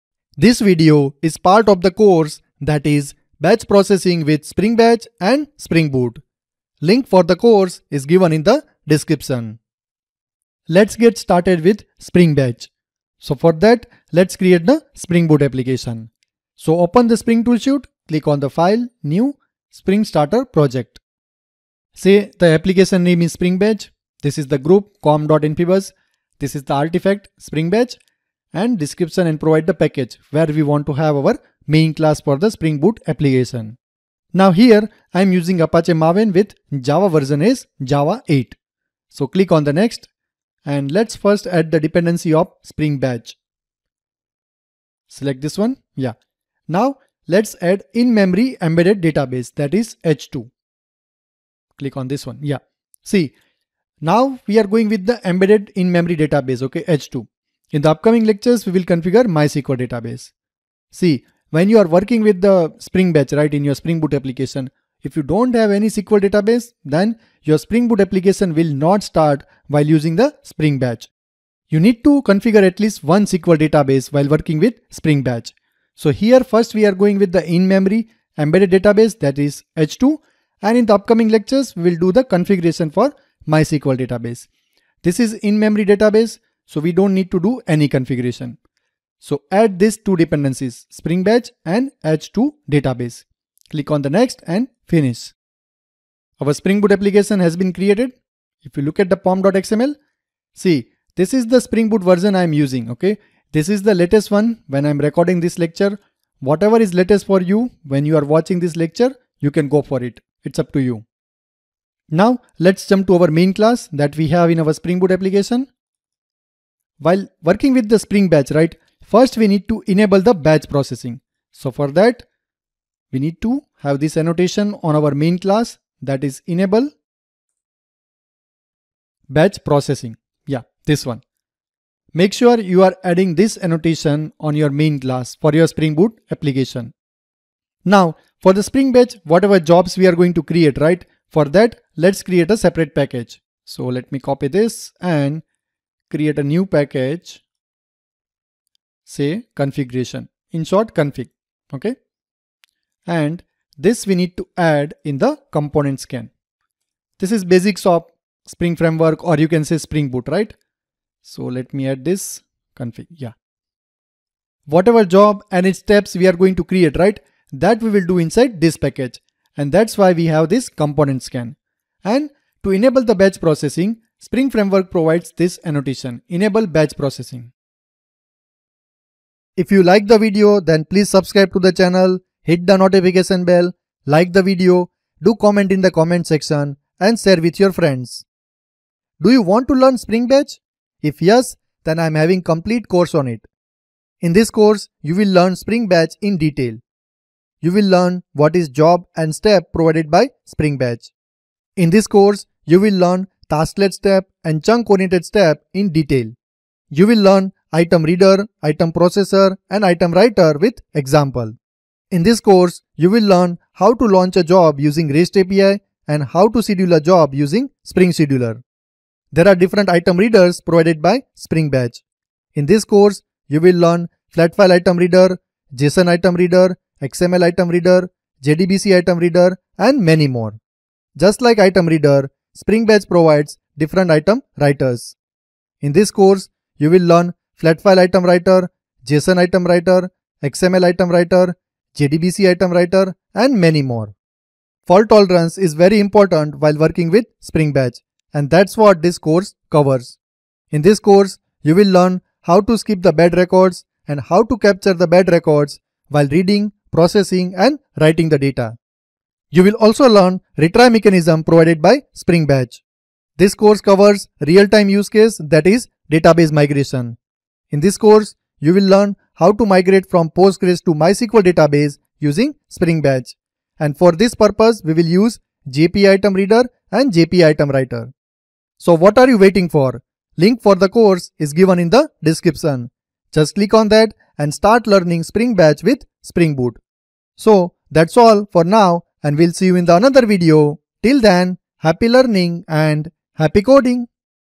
Create Spring Batch Application
In this video we will Spring Batch Architecture. You will understand Spring Batch Architecture with diagram.

Checkout below Courses Provided by me :-

Learn MongoDB with Java and Spring Boot using Spring Data MongoDB and MongoRepository.